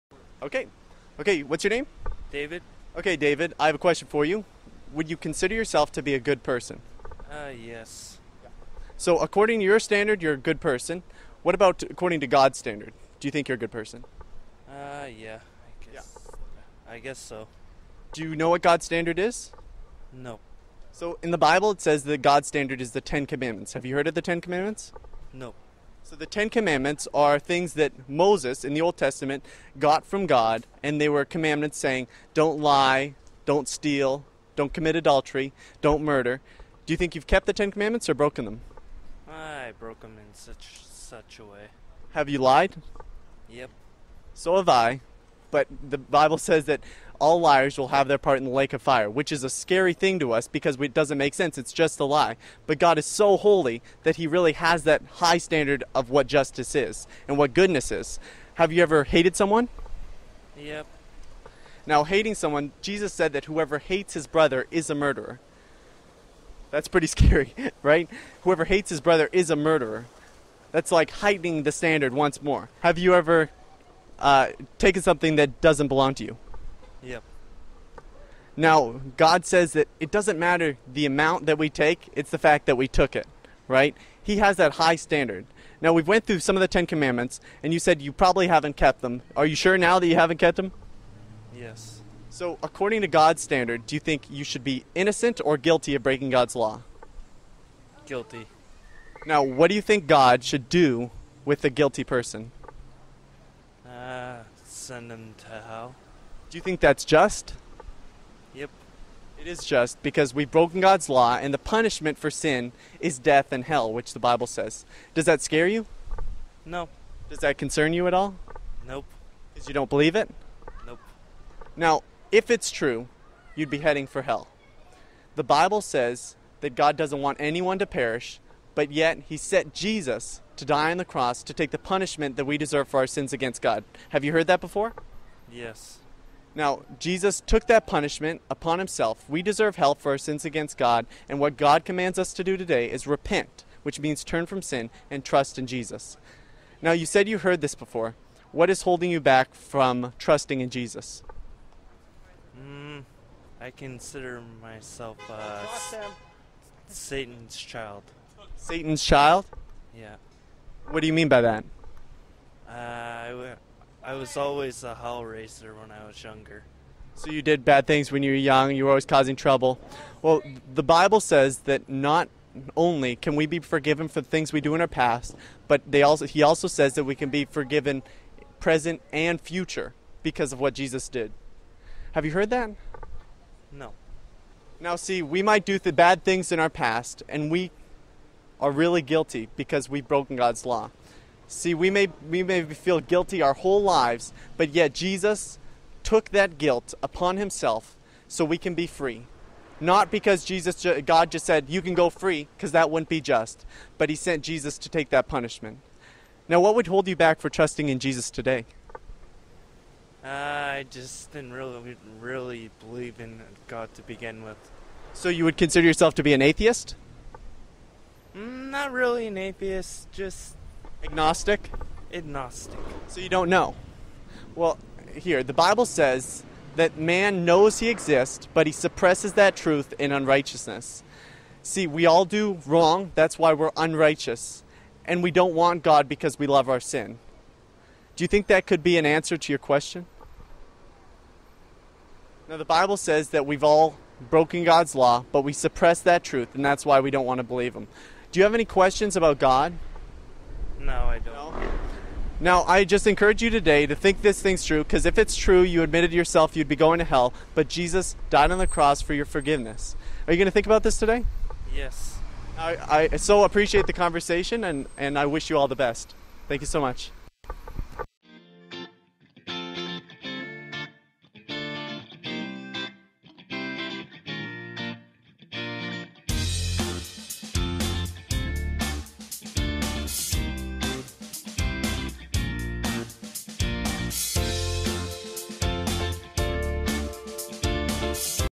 Man claims to be SATAN'S CHILD!: CROSSwalk Talk: episode 5
This week Isaac talks with someone who claims to be a child of SATAN!!